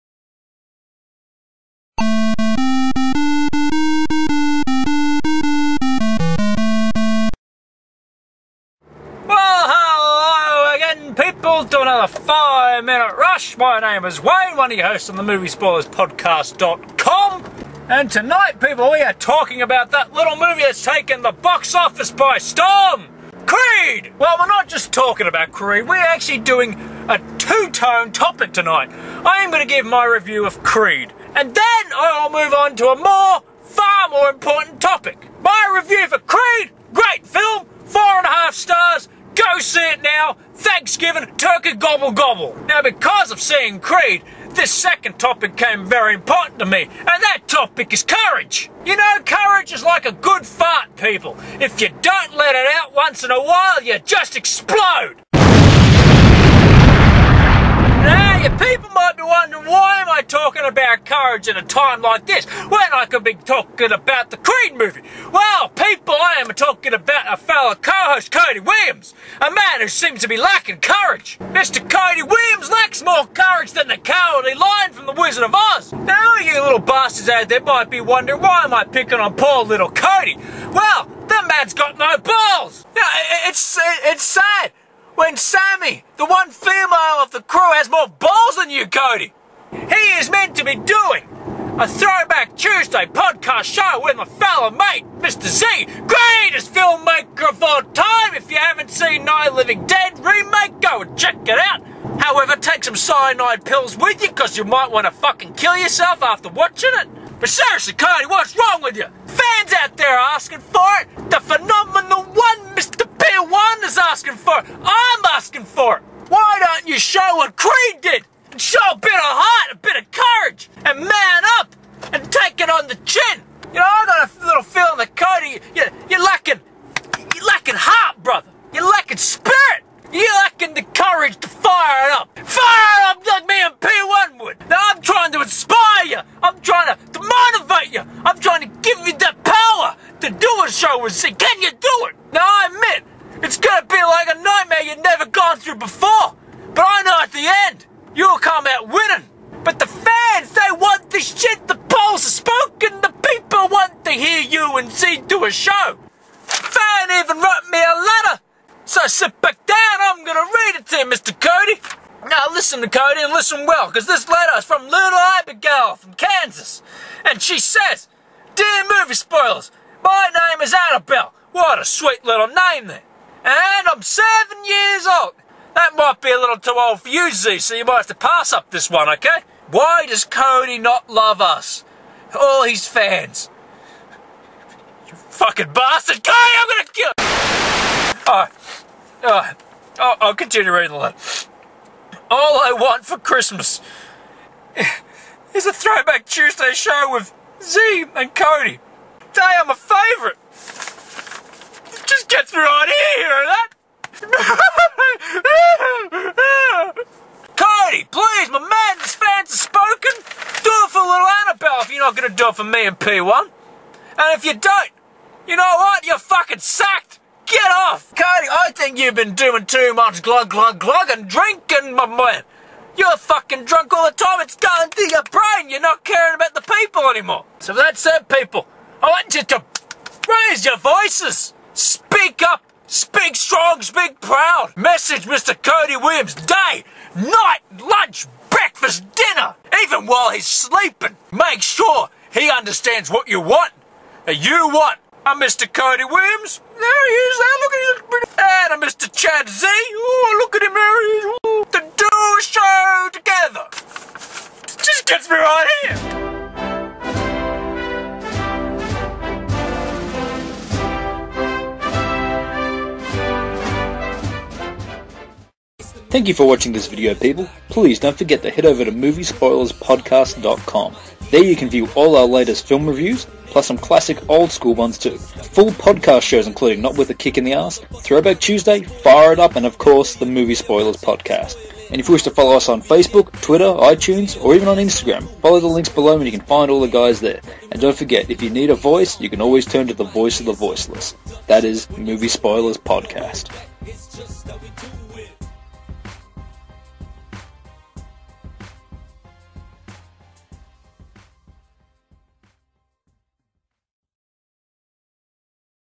5 Minute Rush Courage
WARNING CONTAINS STRONG LANGUAGE: The 5 Minute Rush is a segment that has Wayne talk for 5 minutes about any random topic, this show its I talk about CREED and Khody lacking Courage

twitter:  @wayneasmale   @KhodyW  @MovieSpoilerPod @SBulldog99